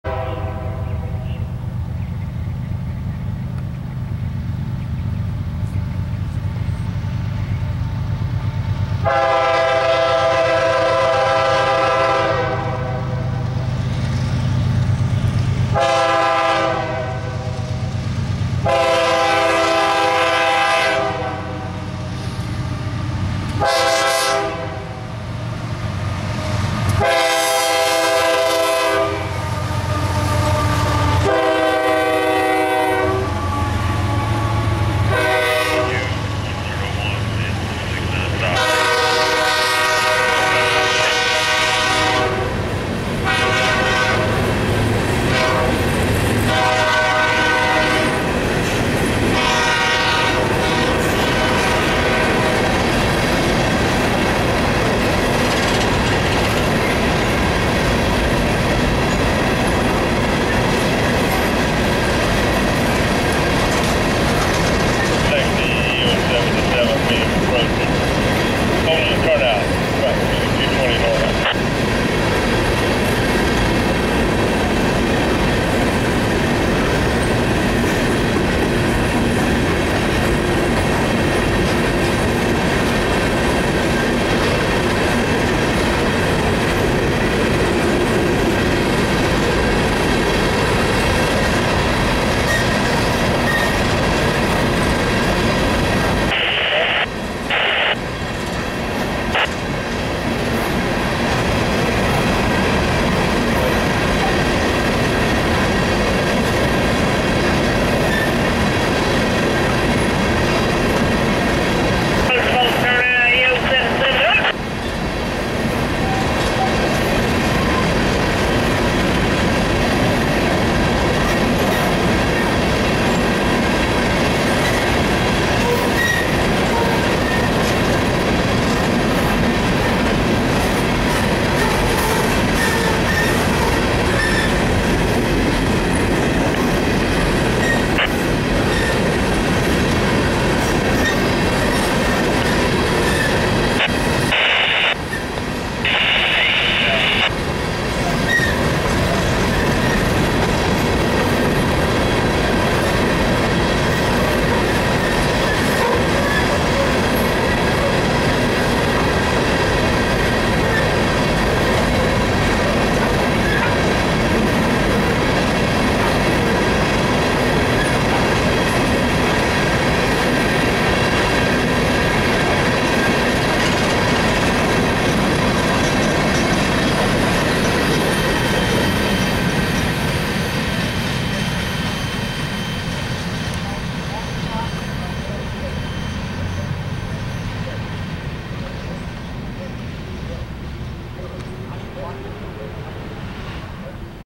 CSX 220 leads the CSX U101 at Folkston
CSX 220 (GE AC44CW ) CSX 3128 ( Ex LN GE B23-7)and CSX 33 (GE AC44CW) lead the CSX U101 at Folkston on 5/11/2014 at 3:28 pm